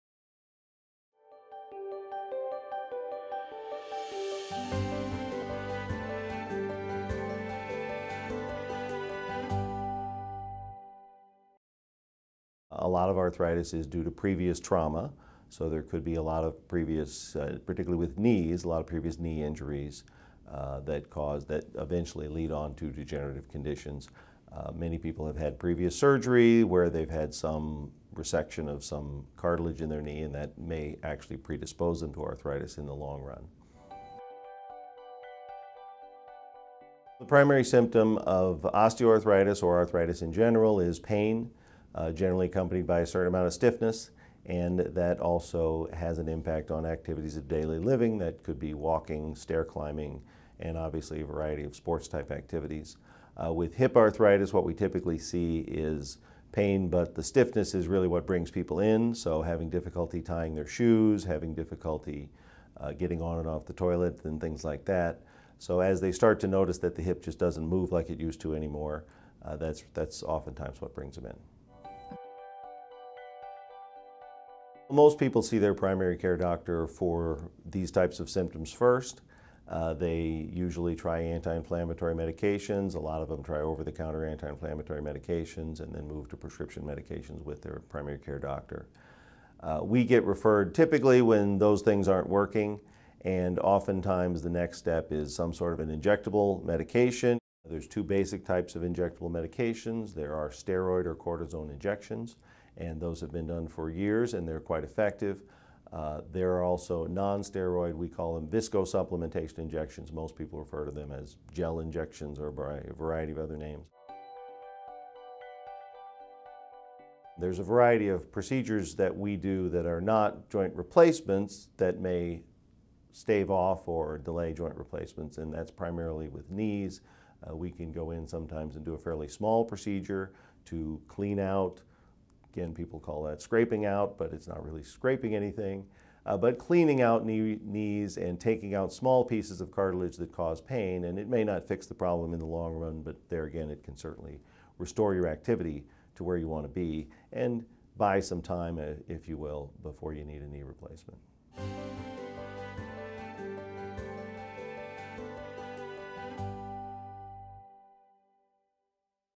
Osteoarthritis: Causes, Symptoms, Medication Options
Douglas Murphy, MD, orthopedic surgeon, Holy Cross Germantown Hospital describes the causes and symptoms of osteoarthritis. Dr. Murphy also discusses medication options and other non-surgical treatments for this condition.